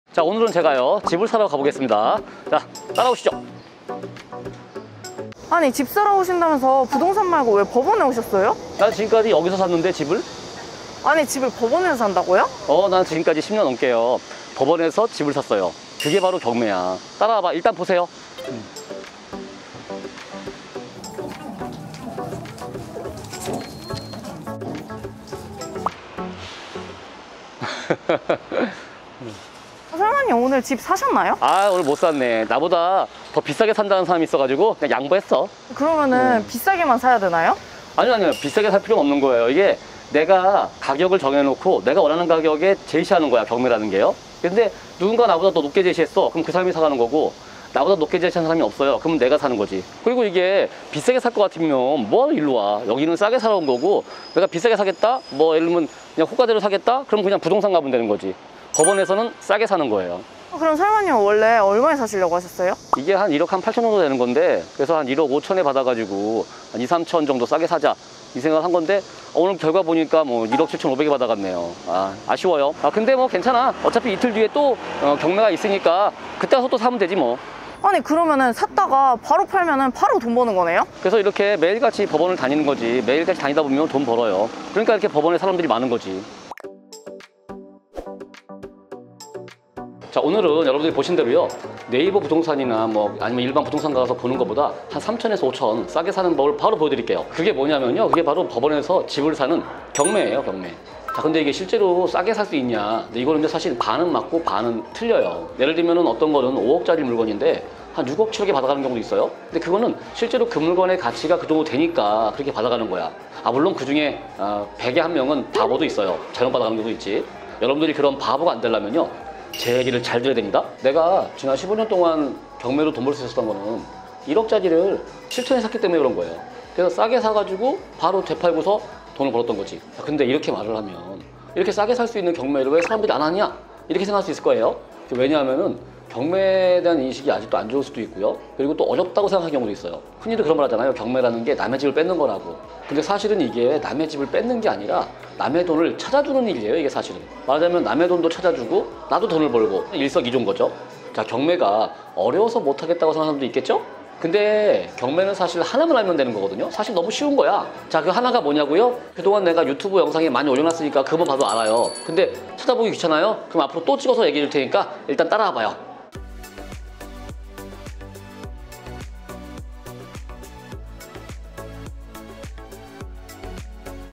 내 집 마련, 전세 가격으로 집을 살 수 있다고? 집값이 떨어져도 괜찮음!
쉽게 알려주는 부동산 경매 강의. HOME336 대표 설마쌤을 영상으로 만나봅시다

💰 설마 안정일 경매강의 기초반/심화반/실전반 💰
✔️ 카카오톡 친구추가    @home336lab